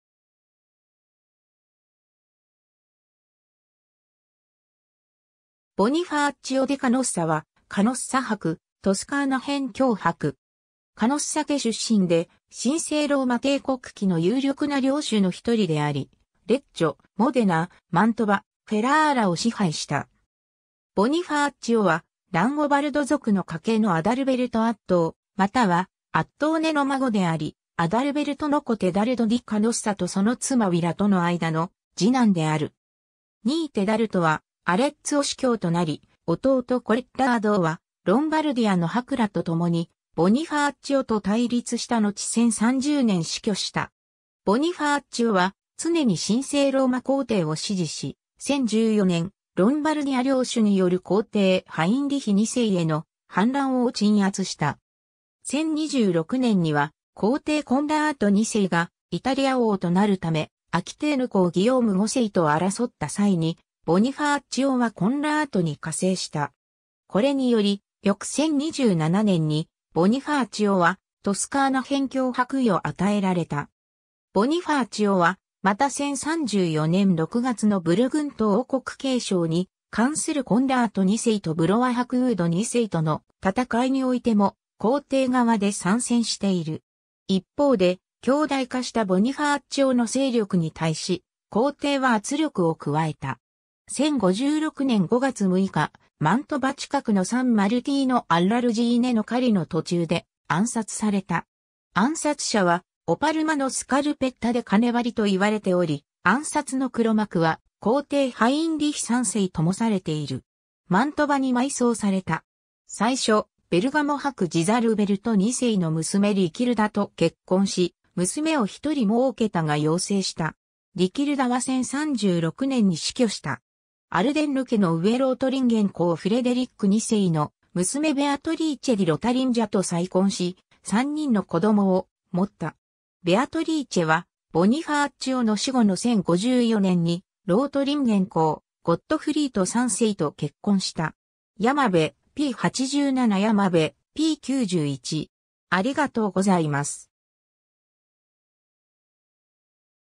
ボニファーチオ・ディ・カノッサ
ボニファーチオ・ディ・カノッサ（Bonifacio di Canossa, 985年 - 1052年5月6日）は、カノッサ伯（在位：1012年頃 - 1052年）、トスカーナ辺境伯（4世）（在位：1027年 - 1052年）。
カノッサ家（Canossiana）出身で、神聖ローマ帝国期の有力な領主の一人であり、レッジョ、モデナ、マントヴァ、フェラーラを支配した。
 ボニファーチオは、ランゴバルド族の家系のアダルベルト・アットー（Adalberto Atto）またはアットーネ（Attone）の孫であり、アダルベルトの子テダルド・ディ・カノッサとその妻ウィラ（ボゾン家トスカーナ辺境伯の出身ともされる）との間の次男である。
兄テダルトはアレッツォ司教となり、弟コッラードはロンバルディアの伯らとともにボニファーチオと対立したのち1030年死去した。
 ボニファーチオは常に神聖ローマ皇帝を支持し、1014年、ロンバルディア領主による皇帝ハインリヒ2世への反乱を鎮圧した。
1026年には、皇帝コンラート2世がイタリア王となるためアキテーヌ公ギヨーム5世と争った際に、ボニファーチオはコンラートに加勢した。
これにより翌1027年にボニファーチオはトスカーナ辺境伯位を与えられた。
ボニファーチオはまた1034年6月のブルグント王国継承に関するコンラート2世とブロワ伯ウード2世との戦いにおいても皇帝側で参戦している。
一方で、強大化したボニファーチオの勢力に対し、皇帝は圧力を加えた。
 1056年5月6日、マントヴァ近くのサン・マルティーノ・アッラルジーネの狩りの途中で暗殺された。
暗殺者はオパルマのスカルペッタ・デ・カネヴァリと言われており、暗殺の黒幕は皇帝ハインリヒ3世ともされている。
マントヴァに埋葬された。
 最初、ベルガモ伯ジザルベルト2世の娘リキルダ (Richilda) と結婚し、娘を一人もうけたが夭逝した。
リキルダは1036年に死去した。
 アルデンヌ家の上ロートリンゲン公フレデリック2世の娘ベアトリーチェ・ディ・ロタリンジャ（Beatrice di Lotaringia）と再婚し、3人の子供を持った。
 ベアトリーチェはボニファーチオの死後の1054年にロートリンゲン公ゴットフリート3世と結婚した。
 ^ 山辺、p.87 ^ 山辺、p.91